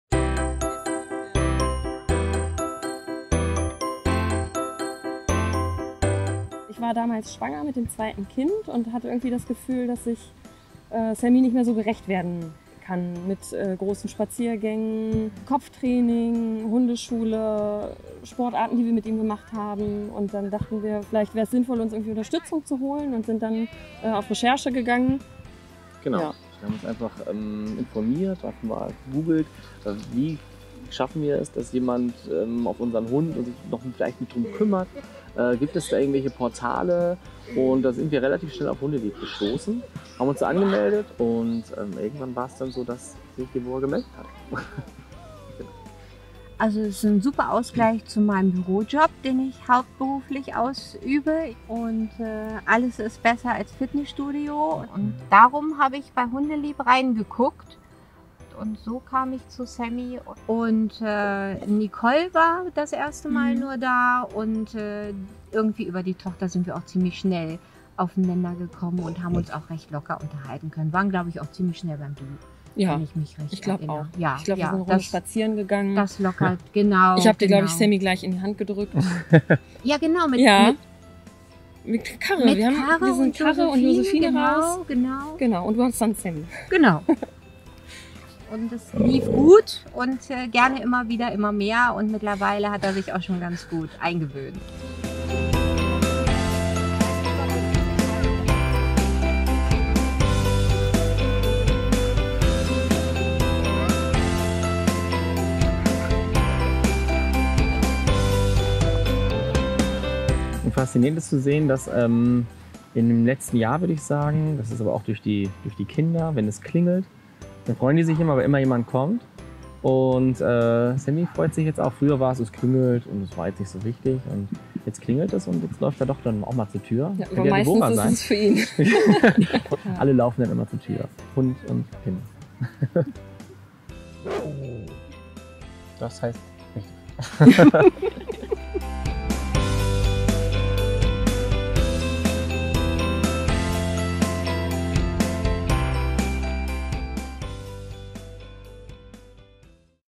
Geteilte Leine: Dogsharing als Ausgleich zum Bürojob
Deborah hat über HUNDELIEB Hund Sammy kennengelernt, mit dem sie regelmäßig Zeit verbringt. So hat sie einen Ausgleich zu ihrem Bürojob und entlastet gleichzeitig die Hundehalter Sebastian und Nicole.

Im größten Dogsharing-Portal im deutschen Sprachraum treffen sich mittlerweile zehntausende Hundefans, bestimmt auch in deiner Umgebung! Schau vorbei: HUNDELIEB.com